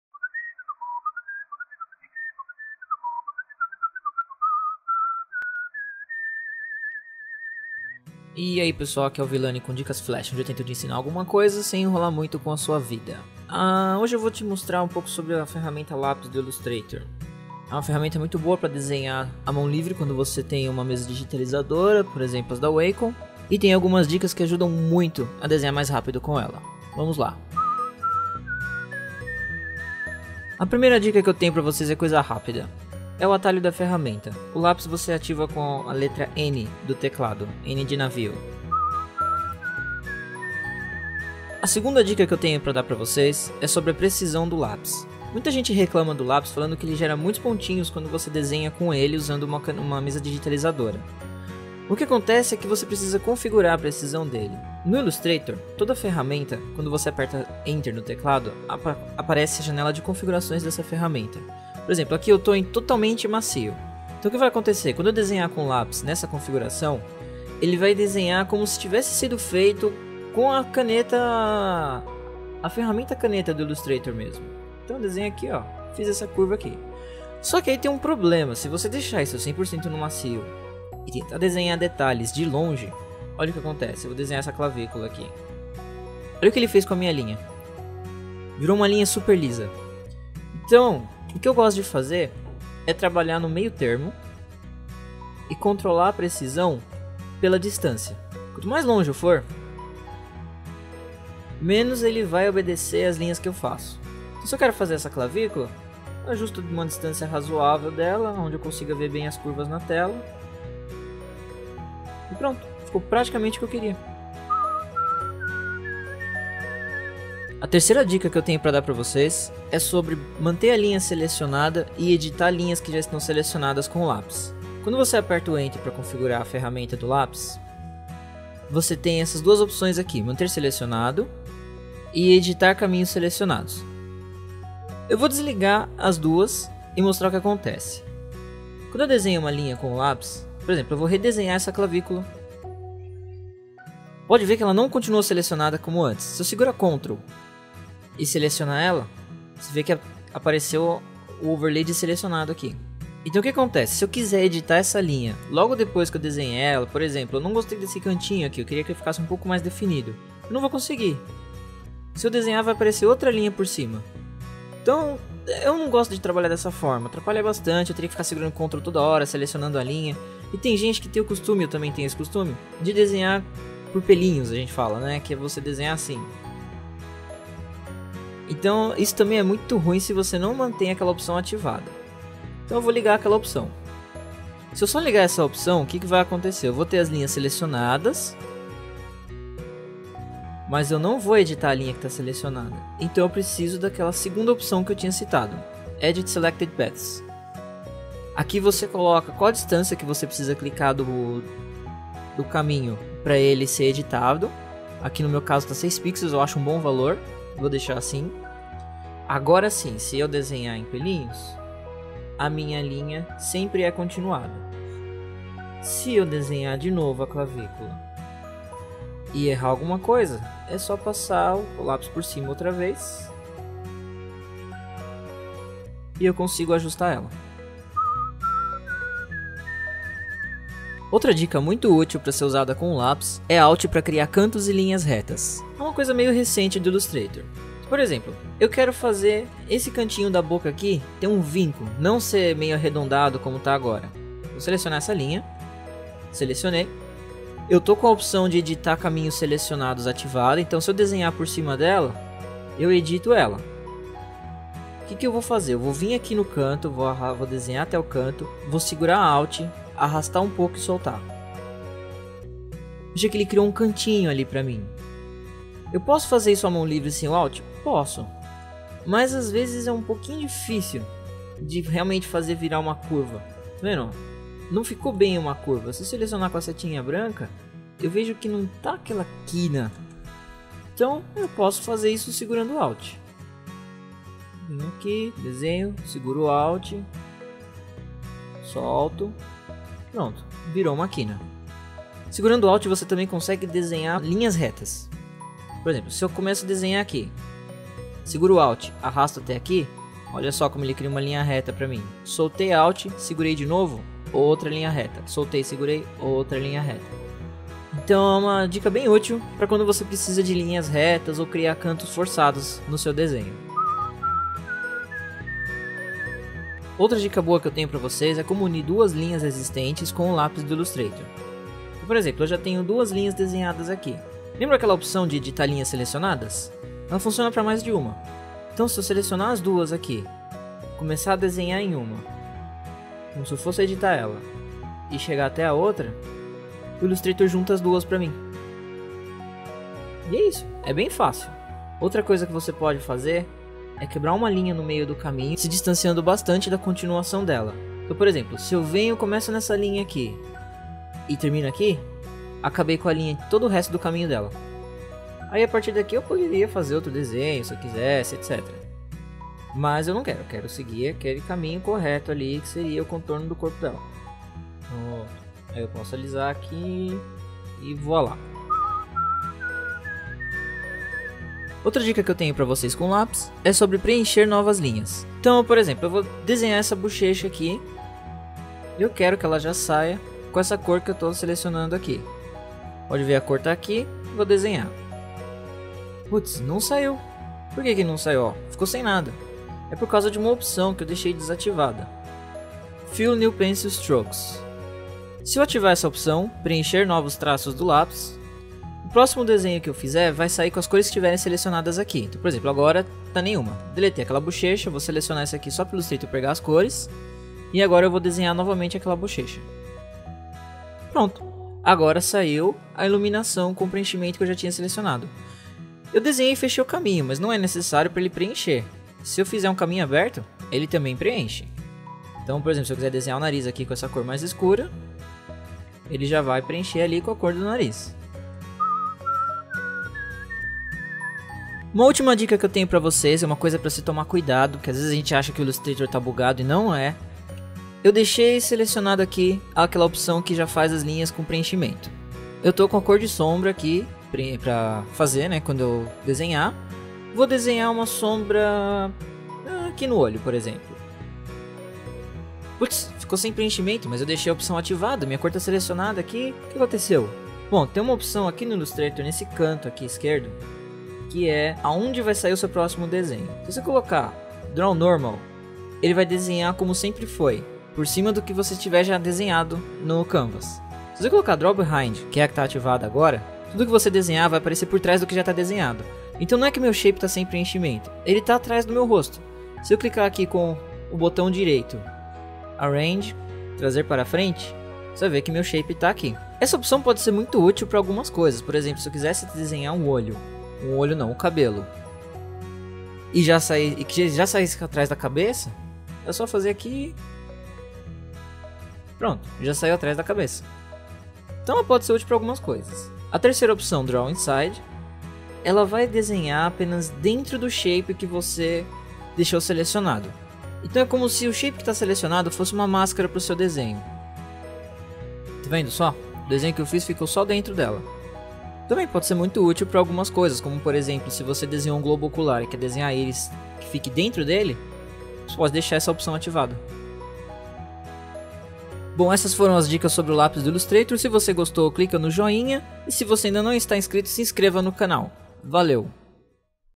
Dicas Flash #1 - O lápis do Illustrator
No primeiro Dicas Flash eu te mostro formas de se trabalhar com o lápis do Illustrator à mão livre.

► Dica 1: atalho (N)
► Dica 2: precisão
► Dica 3: manter e editar linhas selecionadas
► Dica 4: Alt para cantos e linhas retas
► Dica 5: Unir duas linhas existentes
► Dica 6: Colorir novas linhas
► Dica 7: Draw Normal/Behind/Inside